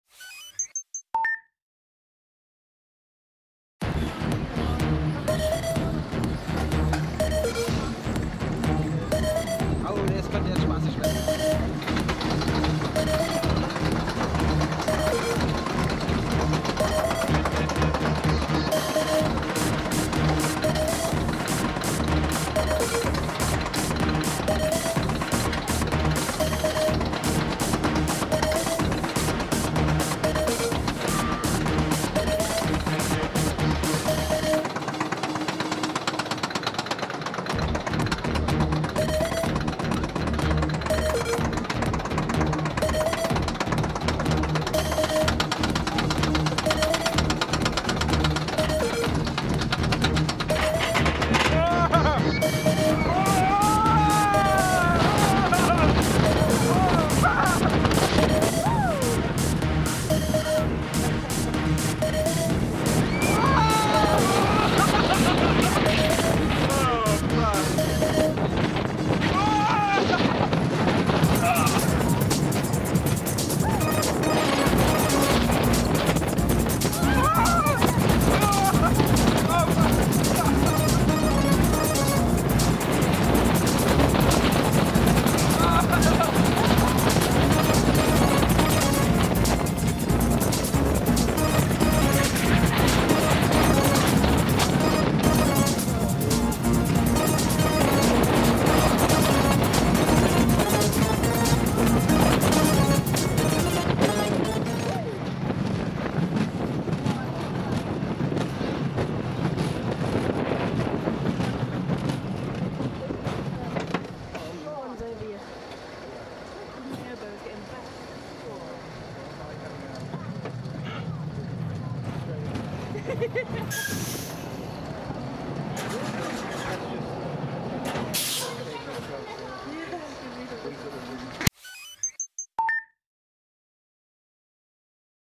Super 8 Achterbahn 360° Mitfahrt/POV ¦ 4K ¦ Wiener Prater ¦ Garmin Virb 360
Hier eine Mitfahrt auf der Super 8 im Wiener Prater, aufgenommen inkl. Sensoren mit der Garmin Virb360! Viel Spaß beim zusehen!

Unbedingt auch mal selbst fahren!

Schreibt doch eurer Feedback in die Kommentare was ihr euch wünscht oder was man verbessern könnte!

Hersteller: Pinfari
Länge: 780 m
Höhe: 21 m